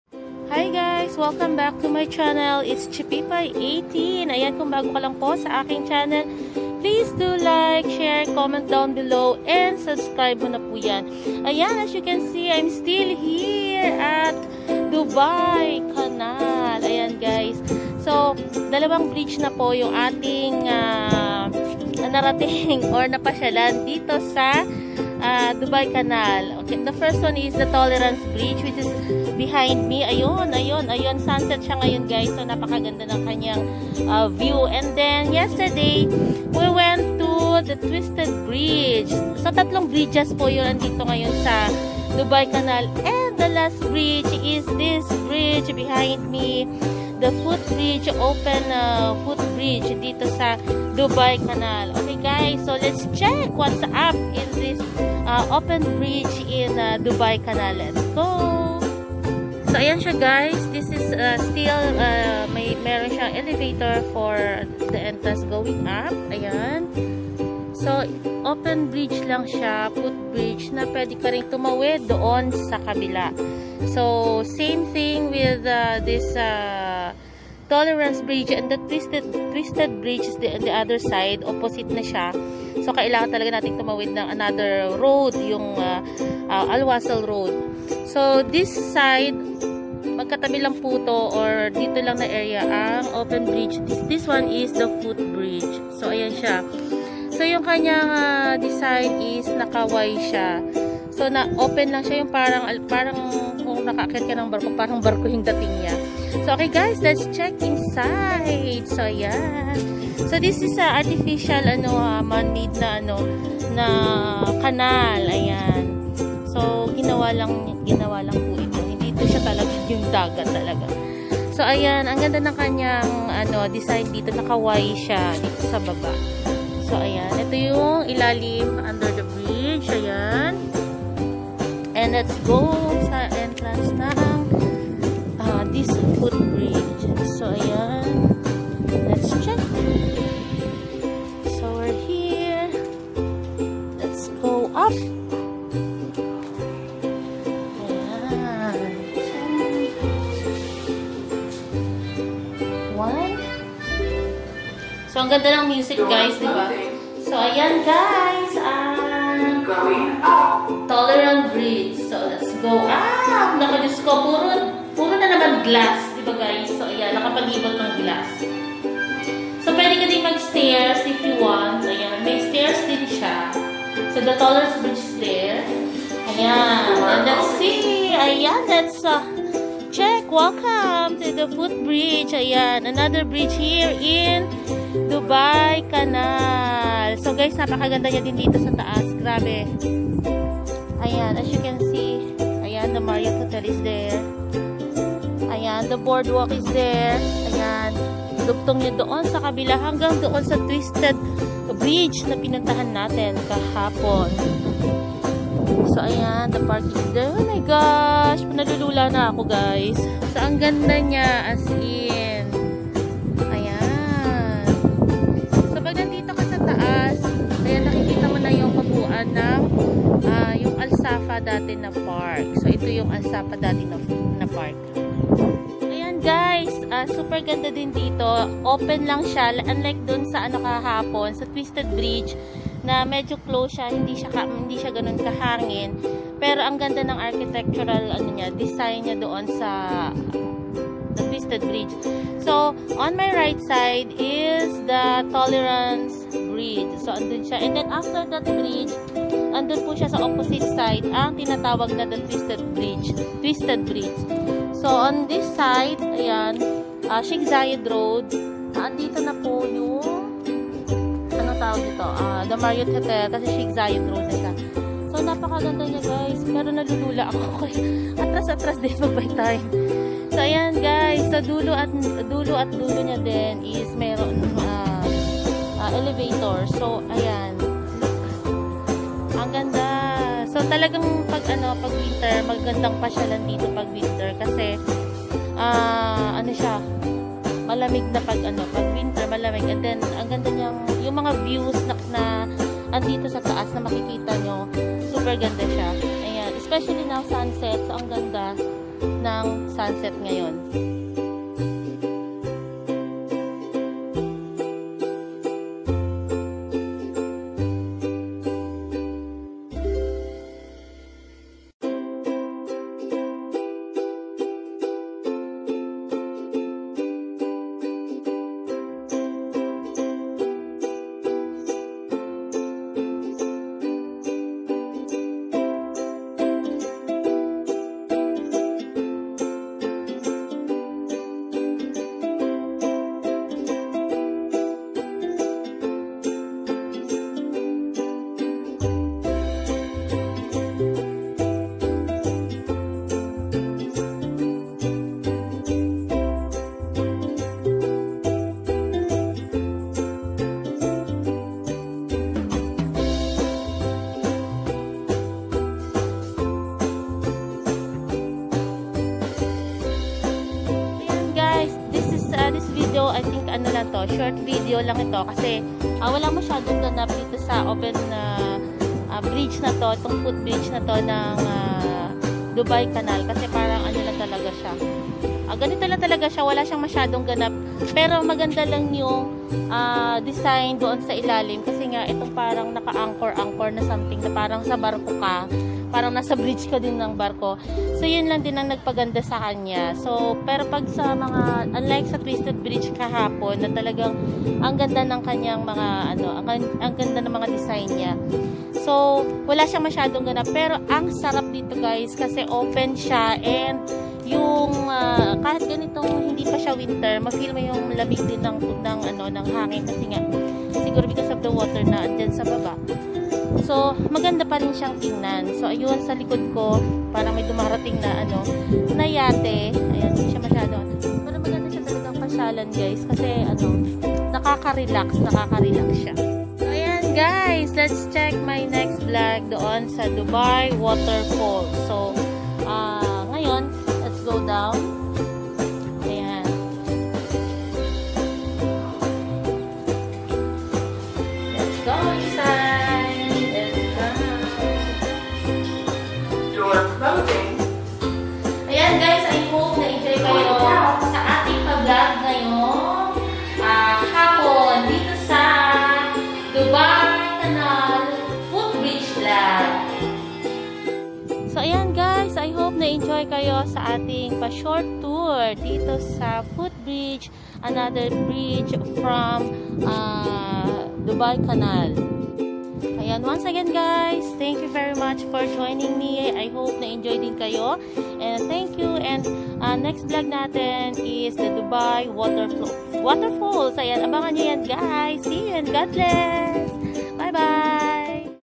FOOT BRIDGE || DUBAI CANAL || CHIPPYPAY18
The first footbridge is a metal bridge suspended by steel cables, with a total span of 120 m (69 m for the main span and 22 m for each of the two side spans) and a deck width of 6 m. The main peculiarity of this bridge is the distinctive Y-shape of the pylons. Because of this pylon shape, the eccentricity between the deck and the upper anchorages gives the catenary effect on the plane view and the hanger cables have an inclined configuration.